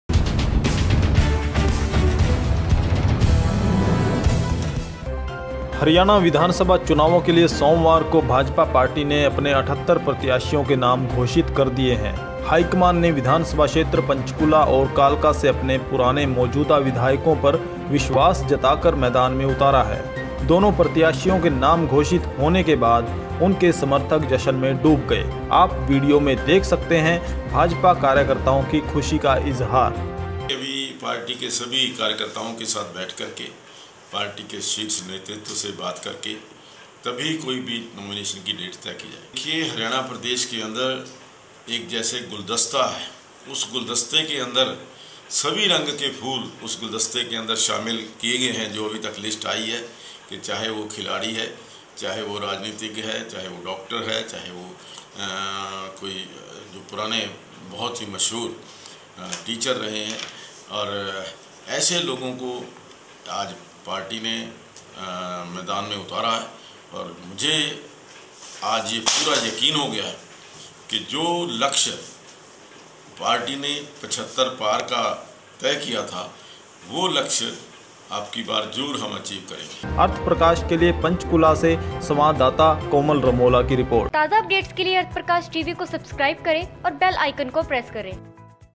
WATCH ज्ञानचंद गुप्ता और लतिका शर्मा को टिकट मिलने पर समर्थकों में जश्न का माहौल l Arth Prakash TV
WATCH ज्ञानचंद गुप्ता और लतिका शर्मा को टिकट मिलने पर समर्थकों में जश्न का माहौल